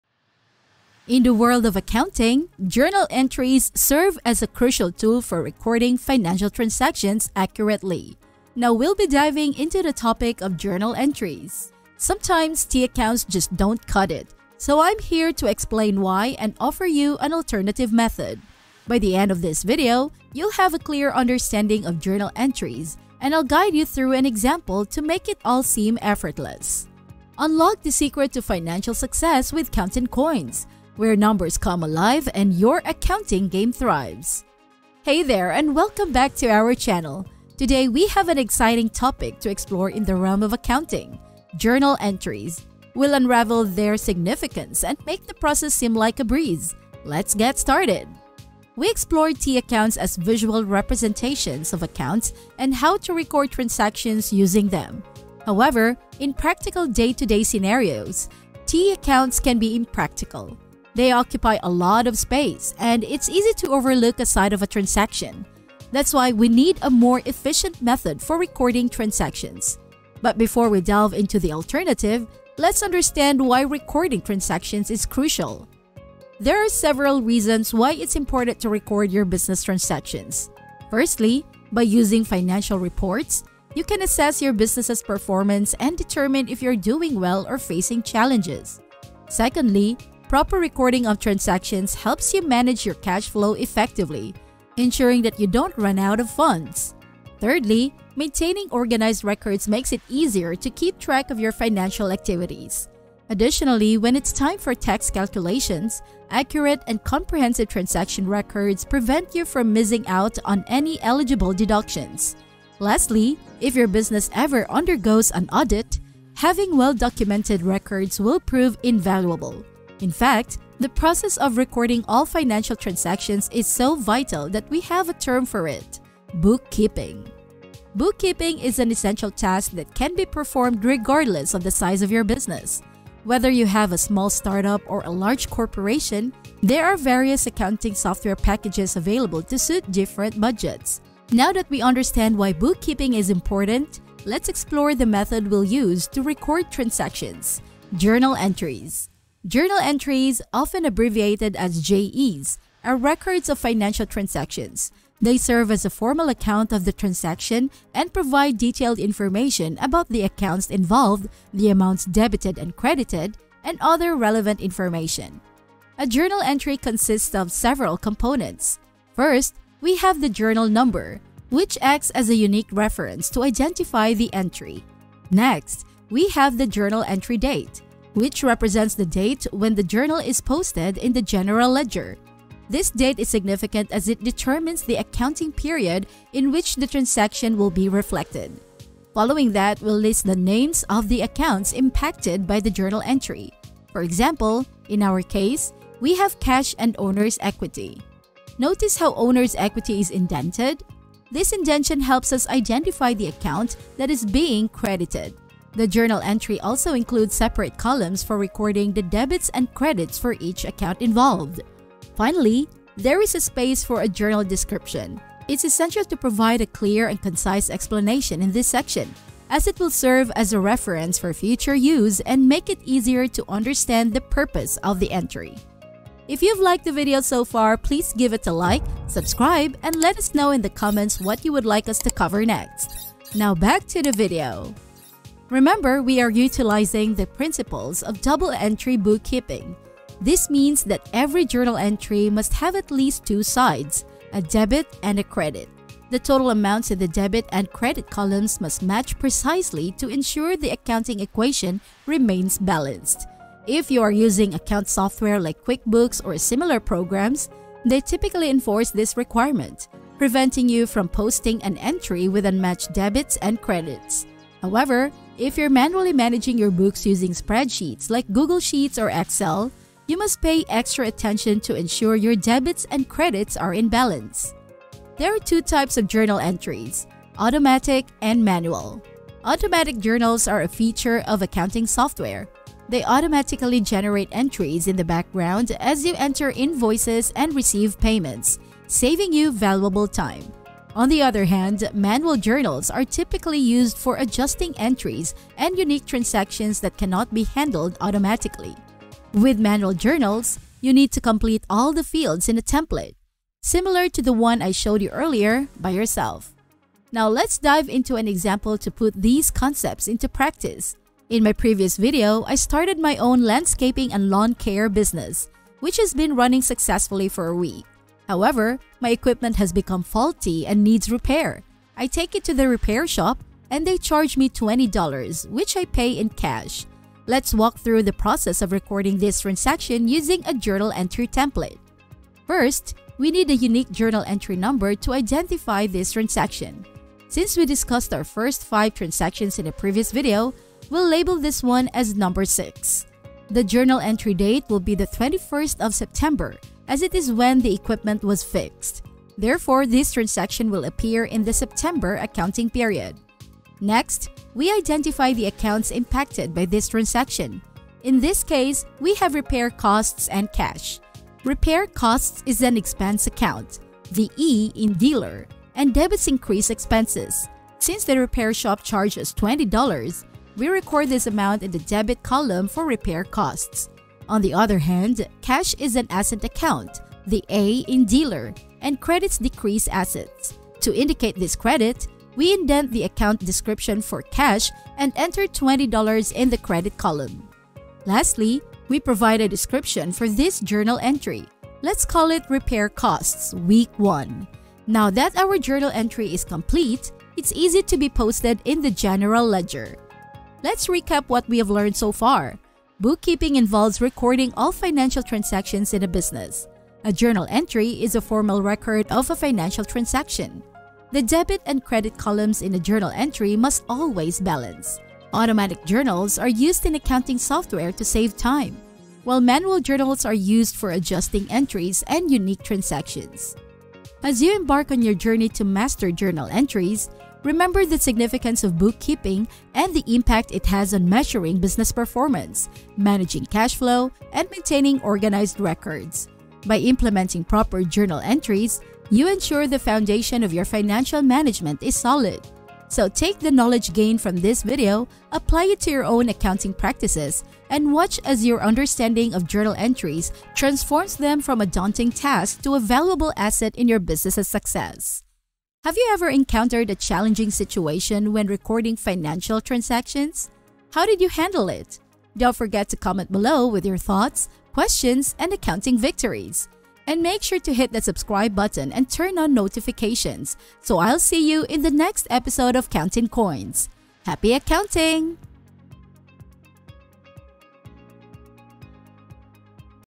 Accounting Basics: How Journal Entries Work
Welcome to another episode of the "Accounting Basics" series. Today, we're diving into the world of Journal Entries. In this tutorial, you'll learn the definition of Journal Entries and their importance in Basic Accounting for Beginners, the most common format for Journal Entries and how to record them, tips on ensuring the balance of your Journal Entrie, and the differences between Manual Journals and Automatic Journals. We'll also go through two illustrative examples of Journal Entries. Plus, we'll simplify Journal Entries for those with limited accounting knowledge.

Get ready to expand your understanding of Journal Entries in a straightforward and concise manner.

Timeline ⌚
00:00 - Intro
00:48 - Understanding the Importance of Recording Transactions
01:16 - Why Record Transactions?
02:08 - Bookkeeping for Any Budget
02:21 - Introducing Journal Entries
02:46 - The Structure of a Journal Entry
03:55 - Balancing Journal Entries
04:35 - Automatic and Manual Journal Entries
05:07 - Journal Entry Example
06:41 - Recap